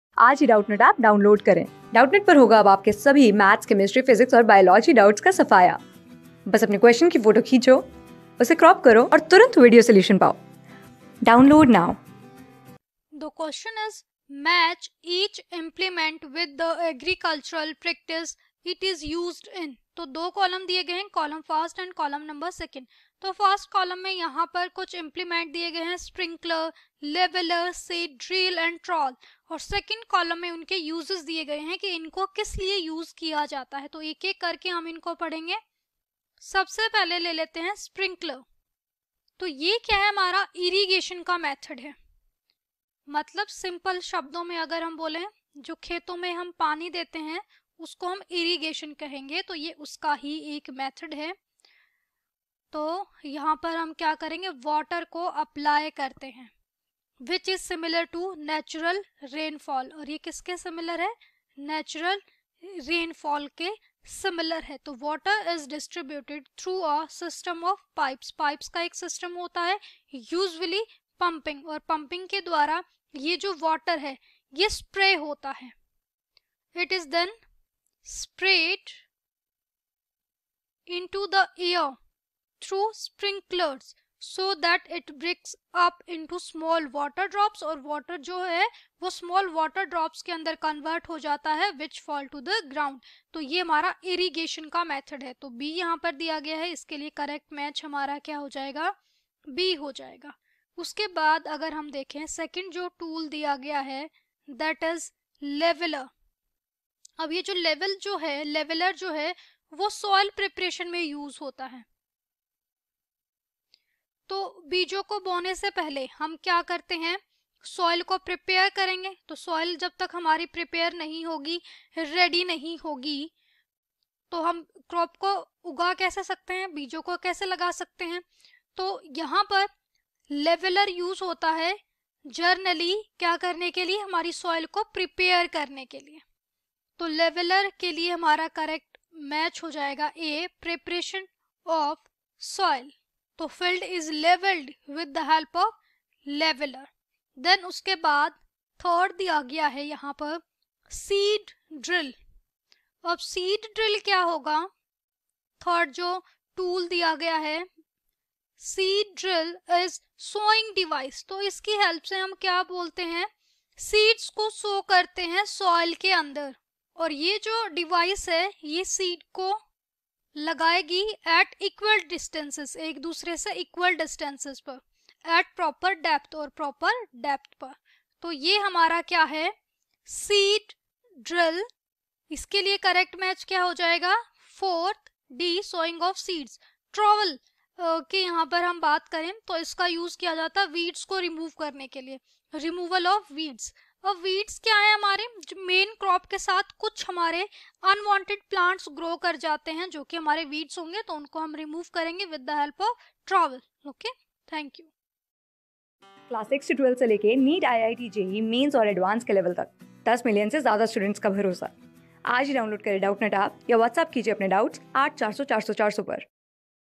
Match each implement with the agricultural practice it is used in. | CLASS 8 | CROP PRODUCTION A...
Match each implement with the agricultural practice it is used in.
Class: 8
Subject: BIOLOGY
Chapter: CROP PRODUCTION AND MANAGEMENT
Board:FOUNDATION

👉 Have Any Query? Ask Us.